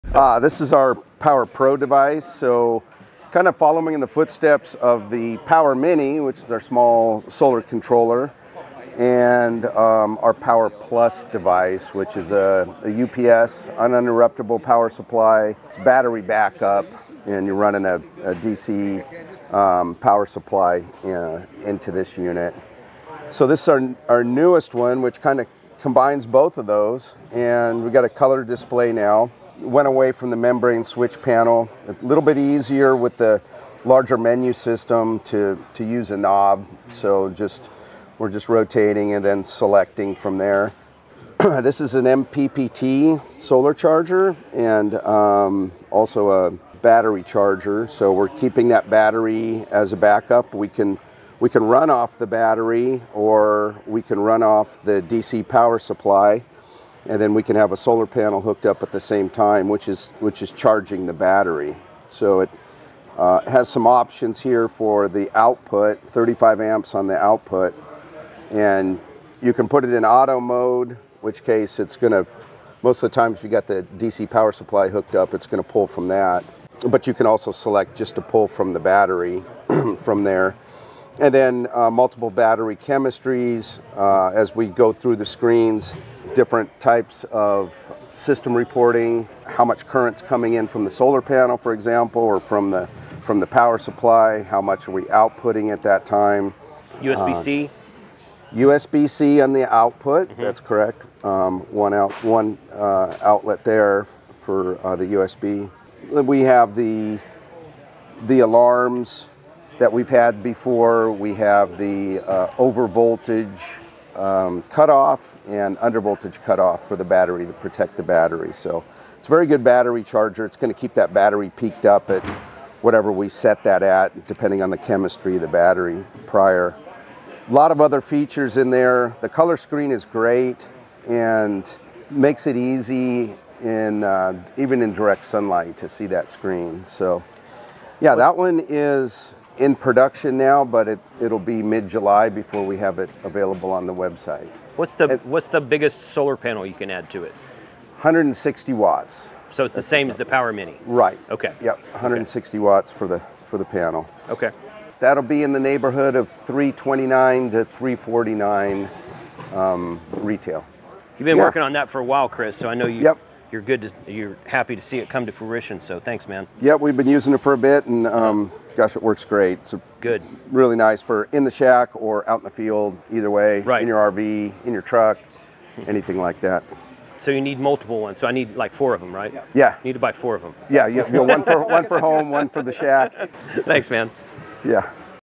Get Ready to MAXIMIZE Your Solar Power with the BuddiPole PowerPro MPPT Controller
Check out the new Buddipole PowerPro MPPT Solar Charge Controller in this YouTube video! Designed for ham radio and off-grid enthusiasts, this innovative DC power management system features a 150W MPPT solar charger, automatic DC/battery switching, and a vibrant LCD display for real-time monitoring. Perfect for portable solar setups, it supports Lead Acid and Lithium-ion batteries with user-friendly controls. Learn how the PowerPro maximizes solar efficiency for your adventures!